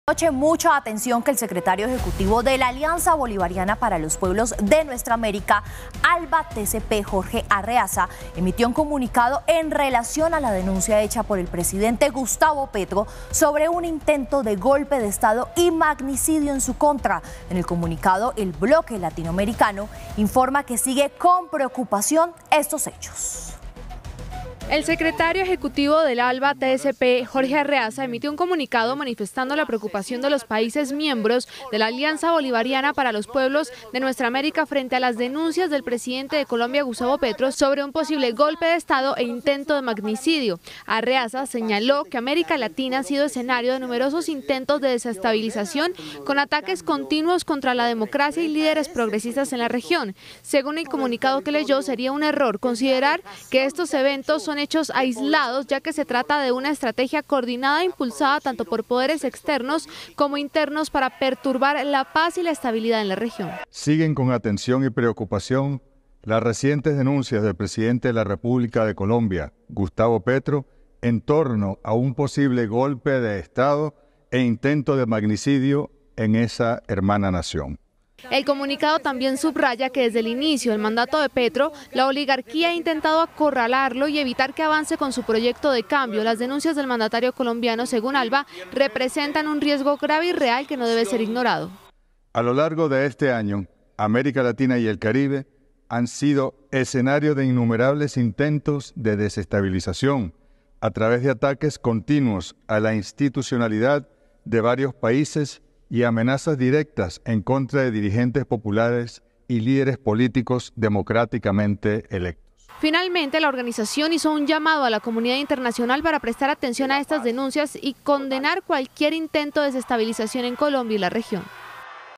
¡Urgente! Comunidad en alerta por denuncias del presidente Gustavo Petro | RTVC Noticias
¡ATENCIÓN! El secretario ejecutivo de la Alianza Bolivariana para los Pueblos de Nuestra América, Jorge Arreaza, emitió un comunicado en relación con la denuncia realizada por el presidente Petro sobre un intento de golpe de Estado en su contra.